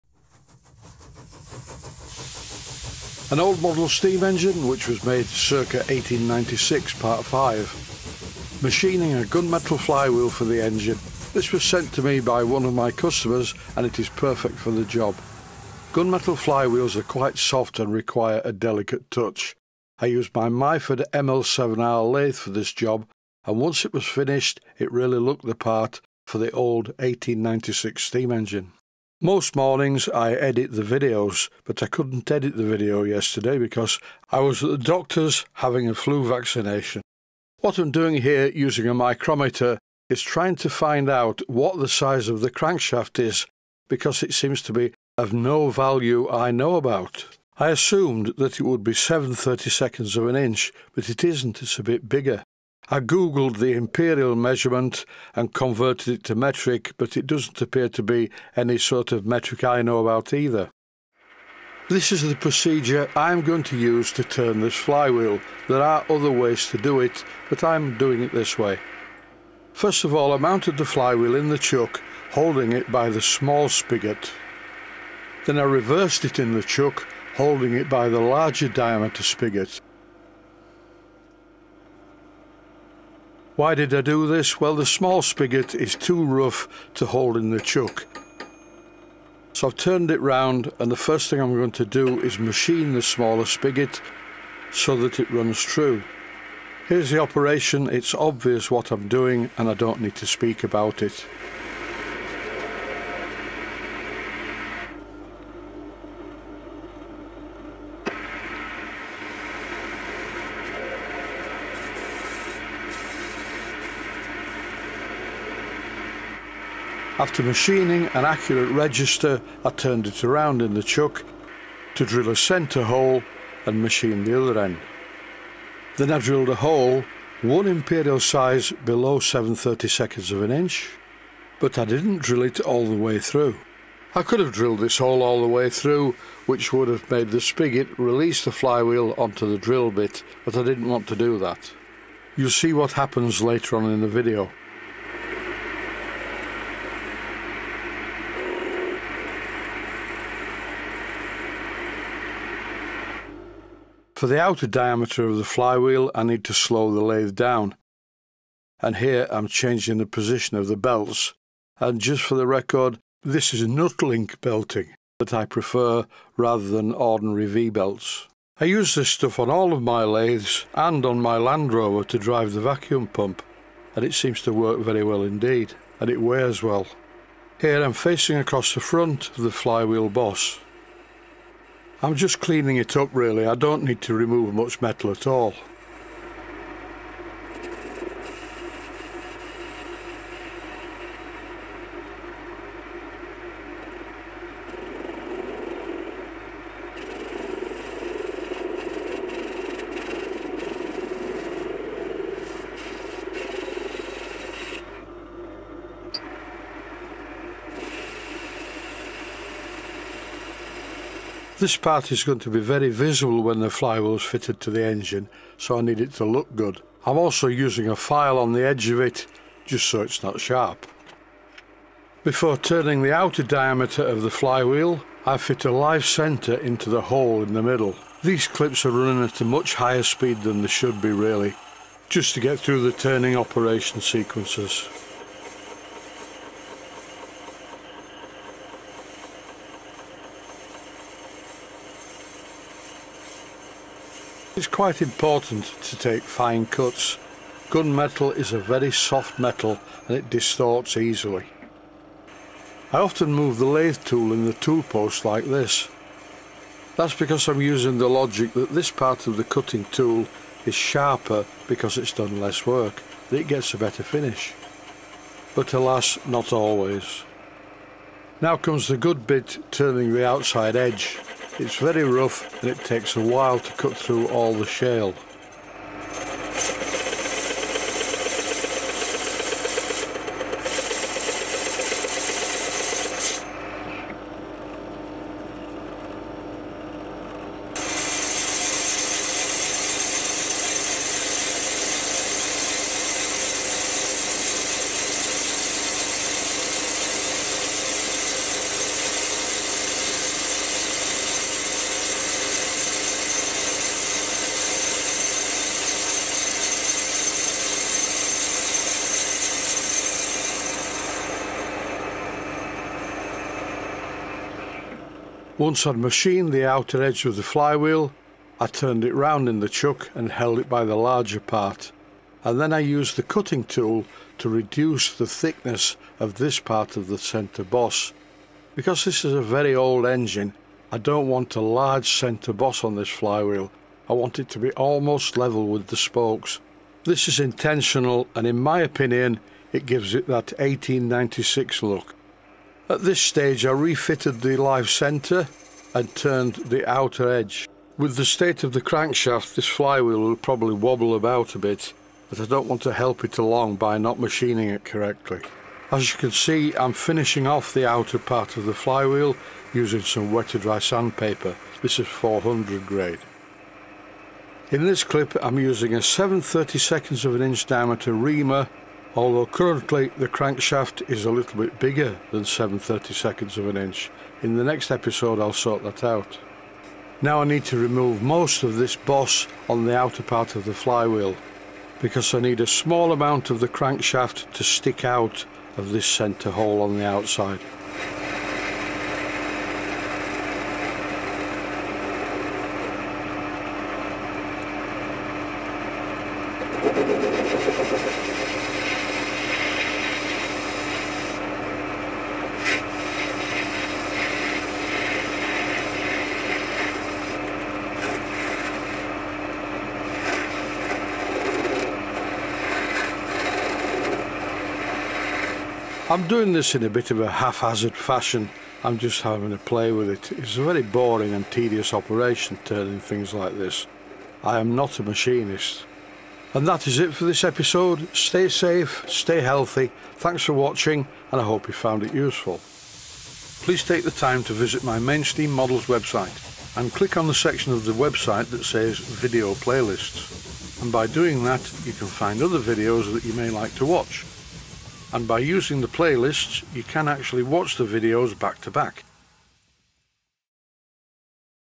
REBUILDING A MODEL STEAM ENGINE MADE CIRCA 1896 - PART #5
Rebuilding A Model Steam Engine Made Circa 1896 - Part #5. Machining a Gunmetal Flywheel for the engine. This was sent to me by one of my customers and it is perfect for the job. Gunmetal Flywheels are quite soft and require a delicate touch. I used my Myford ML7R Lathe for this job and once it was finished it really looked the part for the old 1896 Steam Engine.